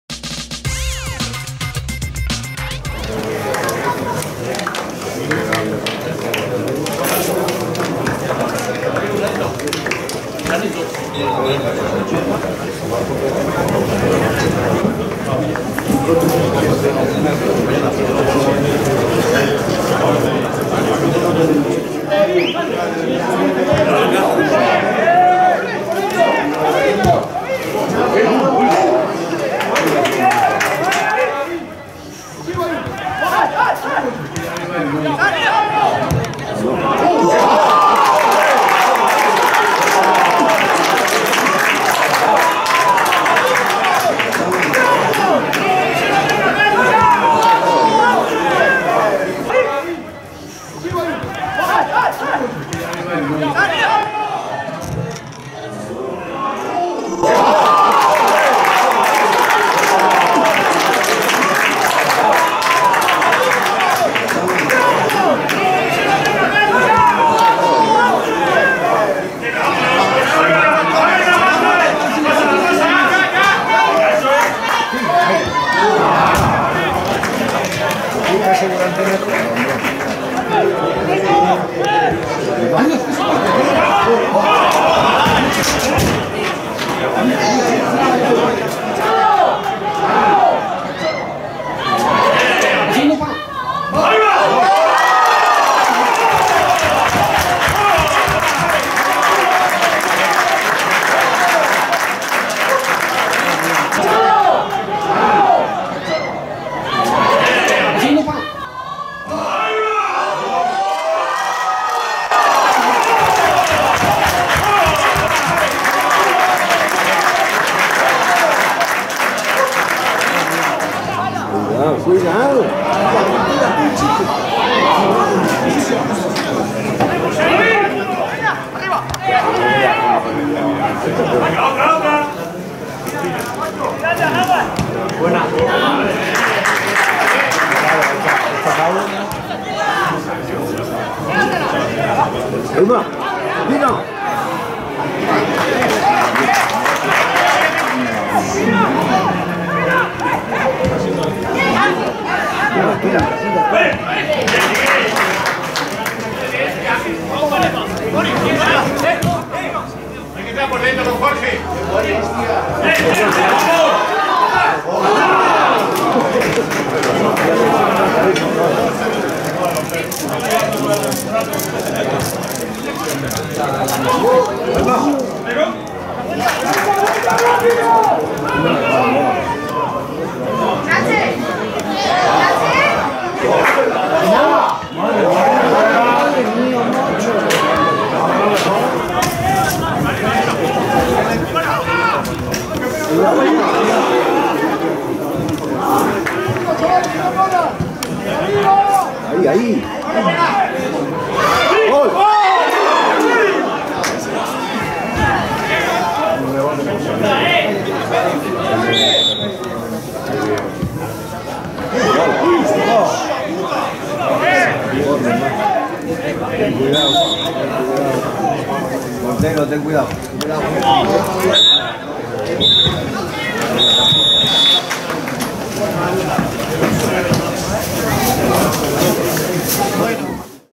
Jornada 25 TEMPORADA 1718 CF INTERNACIONAL DE MADRID - CF POZUELO
Partido: CF INTERNACIONAL DE MADRID 1 - 1 CF POZUELO TOS
Categoria: TERCERA DIVISION
Jornada 25
0 - 1 De Loma (16′)
1 - 1 Lele (39′)